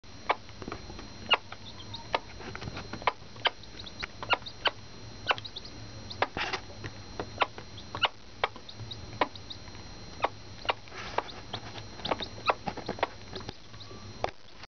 Mama Hen with her babies
The sound of a mother hen alerting her babies to food is distintive and endearing. In this clip, my Silkie hen, Freida shows her surrogate Black Copper Marans chicks where the good stuff is located.